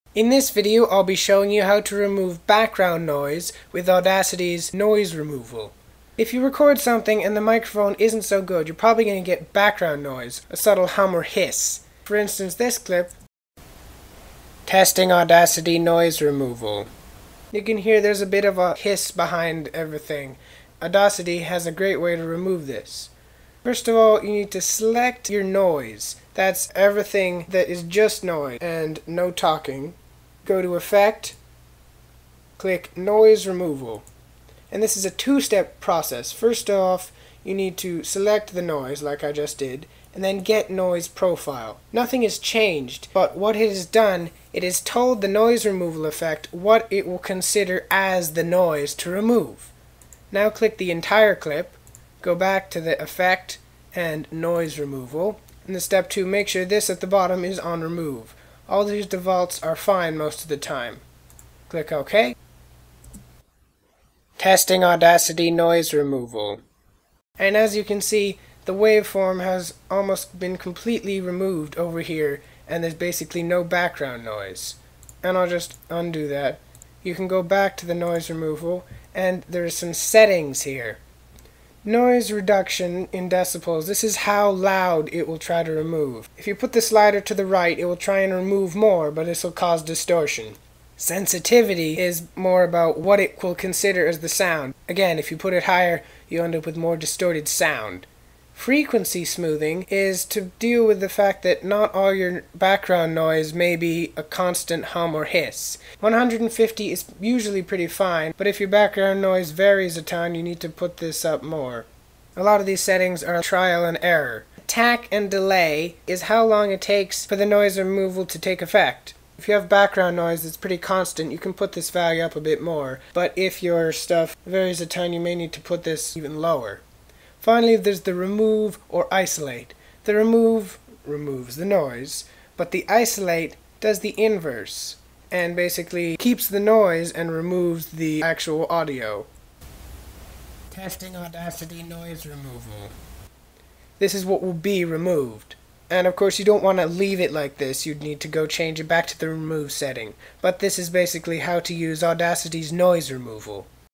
Audacity Noise Removal Tutorial
Recording audio with cheap microphones often picks up background noise, a faint hum or hiss in the background. The best way to get rid of this would be to get better recording equipment, but you can also clean up audio in post using Audacity's Noise Removal.

-Select a part of the clip that only has noise
-Click "Effect" and select "Noise Removal"
-Click "Get Noise Profile"

-Now select the entire clip
-Click "Effect" and select "Noise Removal" again
-Now press "OK" and the noise should be gone

The Noise removal settings are:
-Noise Reduction:  what volume it will remove. Higher value means less background noise, but more chance for distortion
-Sensitivity:  what is considered for noise using gain. Again, higher value means less background noise, but more distortion.
-Frequency smoothing:  Larger the value, the more audio will be considered as "the same".
-Attack/Decay:  How quickly it reacts. Higher value for constant noise, lower value if noise varies quickly.
-Noise:  either "Remove" (remove noise) or "Isolate" (keeps noise and removes everything else).